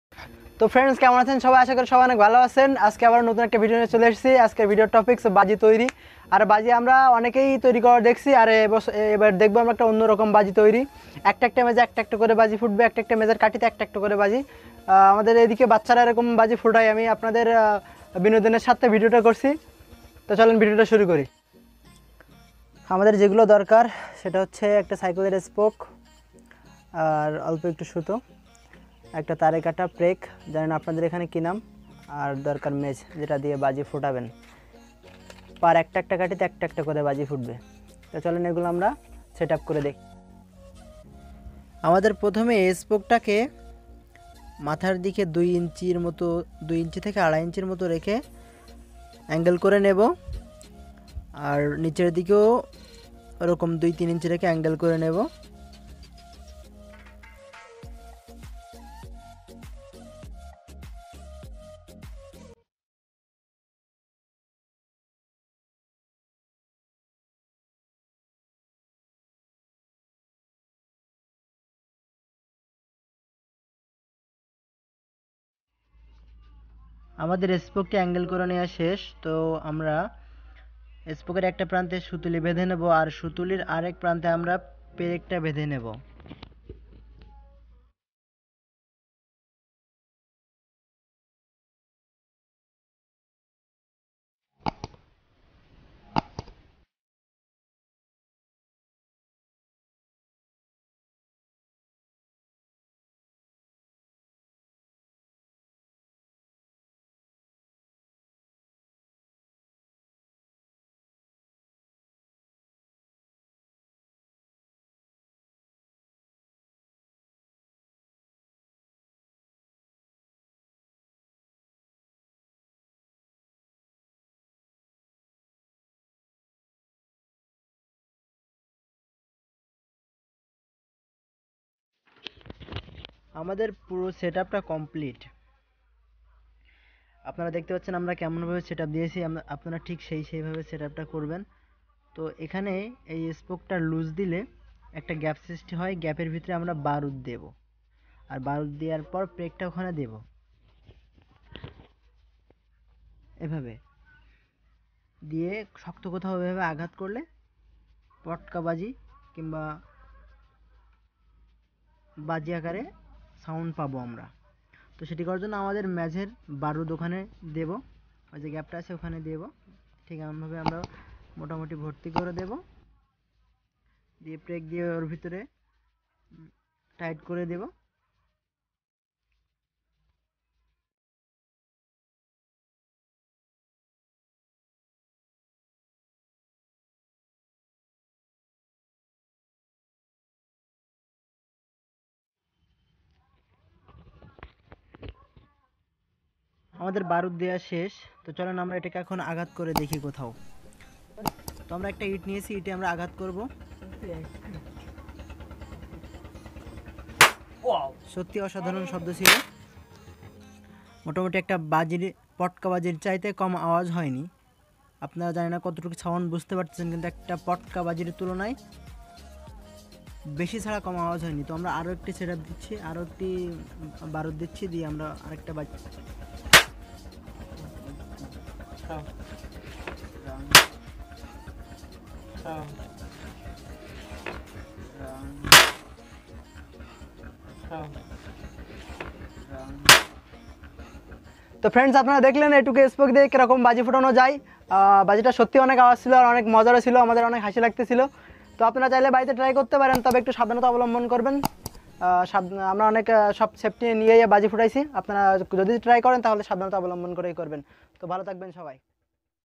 পটকা বাজি তৈরি করা 2 মিনিটে | how to make crackers with bicycle spoke | HEY STUPID
পটকা বাজি তৈরি করা 2 মিনিটে | how to make crackers with bicycle spoke

বন্ধুরা একটি সাইকেলের স্পোক দিয়ে আর মেজের কাঠি দিয়ে কিভাবে অনেকগুলো বাজি ফোটানো যায় সেটি আমরা বিস্তারিত দেখানোর চেষ্টা করেছি আশা করি ভিডিওটা ভাল লাগবে।।

আমাদের এদিকে বাচ্চারা সচারাচার এরকম ধরনের বাজি ফুটায় তাই ভাবলাম আপনাদের কাছে বিষয়টা শেয়ার করি বিষয়টা অত্যন্ত ইন্টারেস্টিং এবং মজাদার ভালো লাগলে অবশ্যই আমাদের সাথে থাকবেন।

Free music link